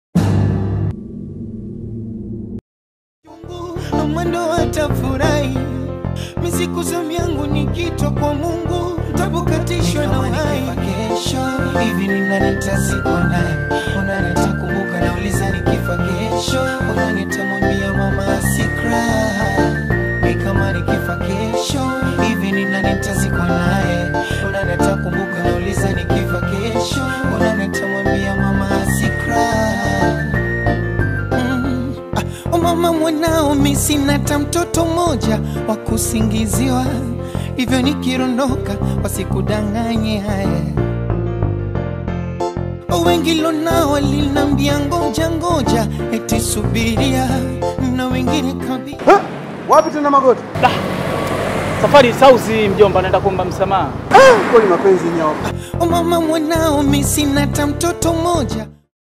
Je una maamuzi kama haya ktk maisha yako ya mahusiano?
Je, wewe ni kijana wa chuo unayesoma na una matarajio mengi katika maisha yako na umeangukia kwenye mitego kama hii? Msomi na mahusiano ipo kwa ajili yako